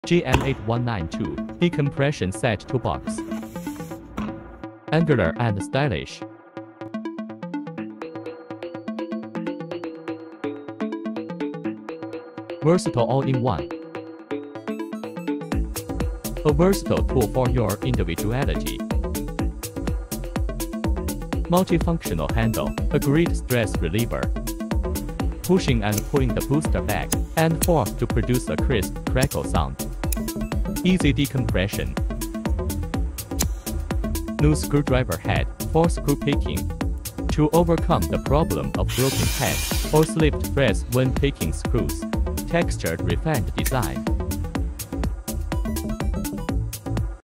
Know more about 180 in 1 CR-V screwdriver bit precision mini toolbox container - JAKEMY JM-8192
The following video will take you in-depth understanding of our product - 180 in 1 CR-V screwdriver bit precision mini toolbox container JM-8192.

*Our product is designed with a focus on enhancing the pursuit of quality of life through a real user experience. The innovative and versatile screwdriver set offers a whole new and fast experience. Our pioneering design breaks away from traditional squared-off tool box models and incorporates a high volume container concept. The modern aesthetics, with automotive inspiration, feature ventilation on both sides for better air circulation in the interior space.
*Our toolbox also features a unique hand toggle action of the hand-operated magnetic box plate, providing a satisfying “pop” sound for a fun and engaging experience. The multifunctional handle with a manually adjustable push slider is designed to adapt to the needs of different palm sizes, making it a great stress reliever. The “booster” produces a crisp “crackle” sound, providing easy decompression by pushing and pulling.
*Additionally, our screwdriver set includes a new screwdriver head for screw picking, overcoming the problem of broken heads or slipped threads when picking screws. It can be used on electric drills and power screwdrivers. With more configuration and less difficulty, our product is designed to make your life easier and more enjoyable.

Keep an eye on our homepage to avoid missing out on new product launches and events!